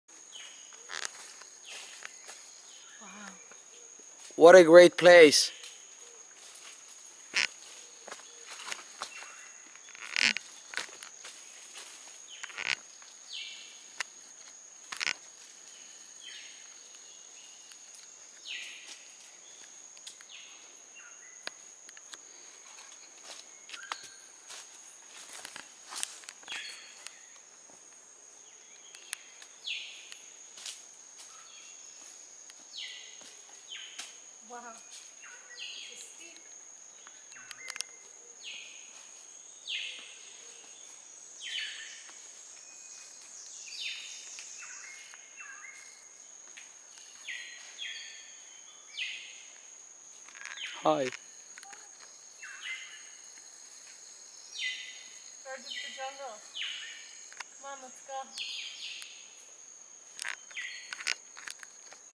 WaituKubuli Trail in Dominica
Dominica is my top favorite island in the Leeward Islands in the Caribbean. Here we took a hike through the rainforest on the Waitukubuli.